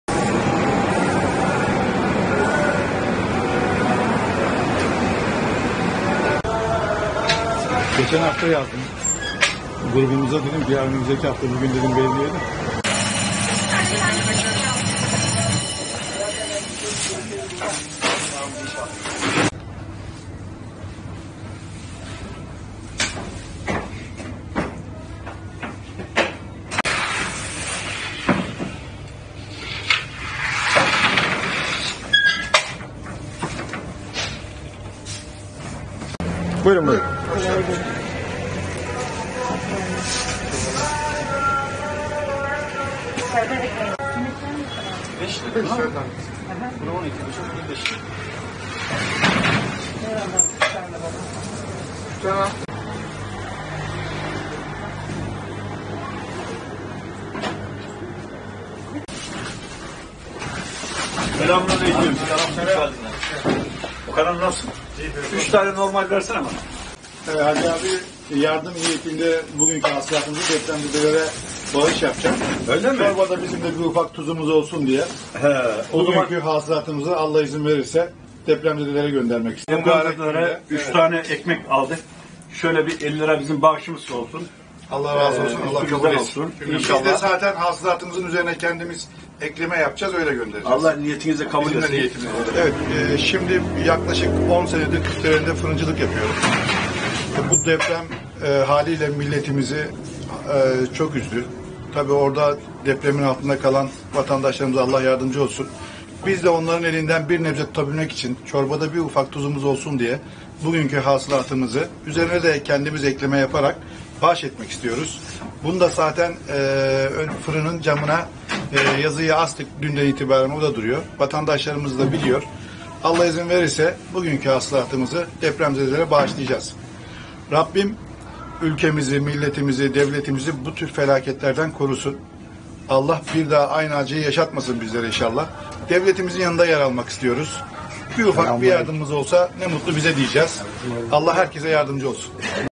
KIRKLARELİ - Trakyalı esnafın deprem bölgesine desteği sürüyor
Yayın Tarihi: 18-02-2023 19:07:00